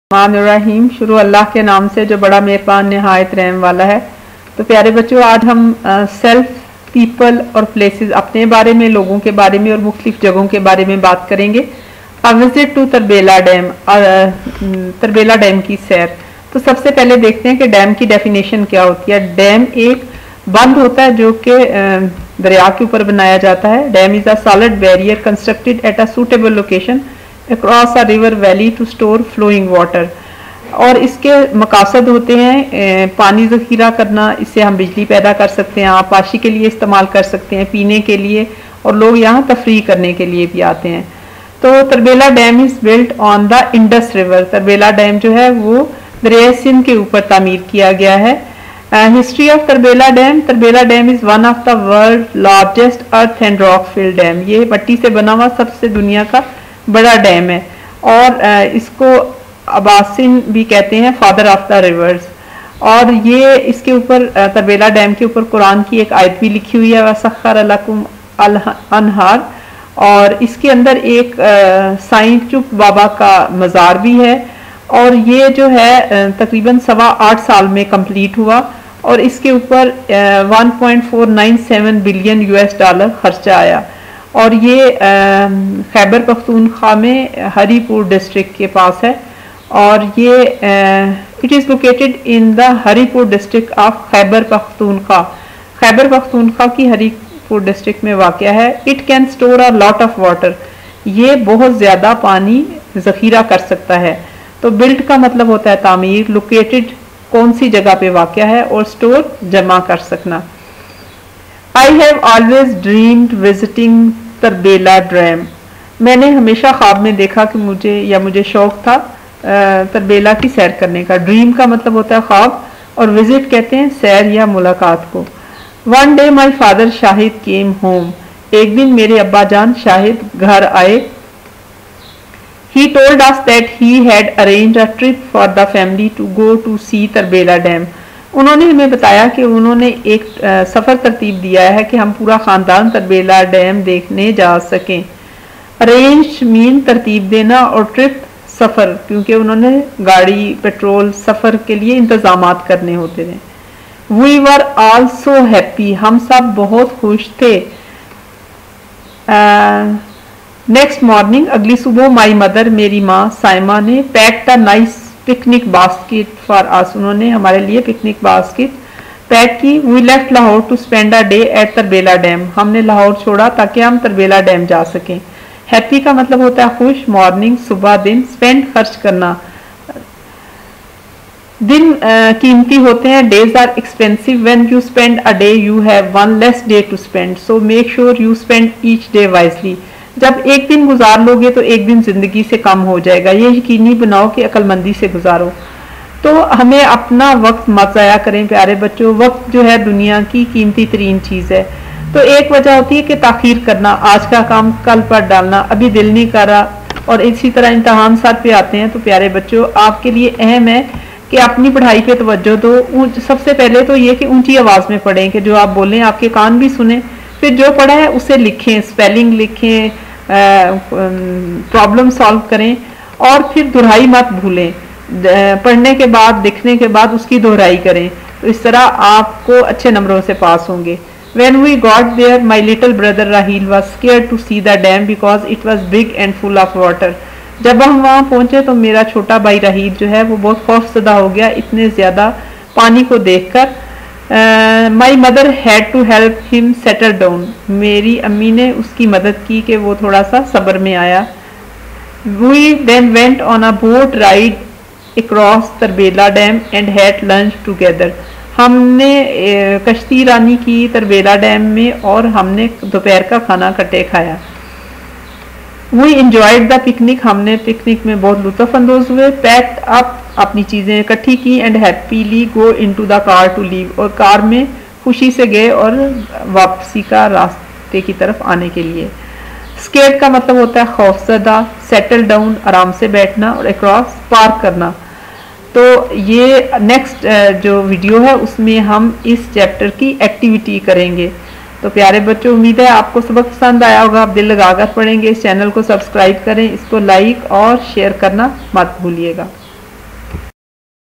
Visit to Tarbela dam/English in Urdu
Class 3 English L 56, story visit to Tarbela dam in Urdu
English story 'A visit to tarbela Dam' for grade 3 kids in urdu